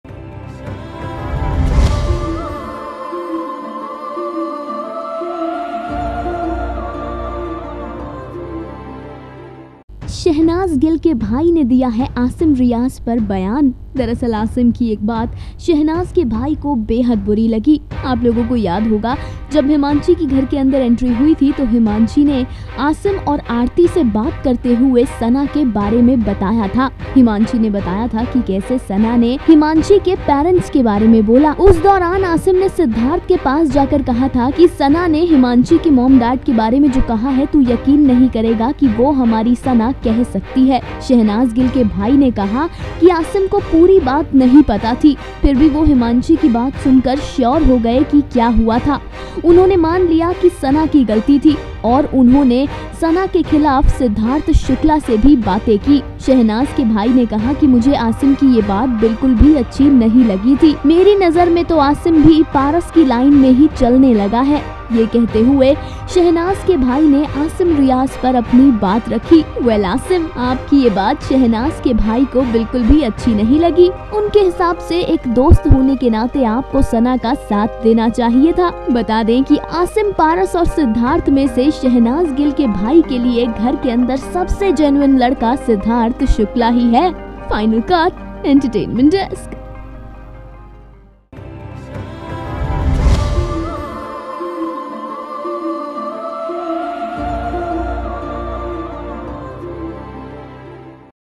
Shehnaaz Gill Brother Thinks Asim & Paras are the Same| Shehnaaz Gill's Brother Shehbaaz
Shehnaaz Gill Brother Thinks Asim ans Paras are the Same| Shehnaaz Gill's Brother Shehbaaz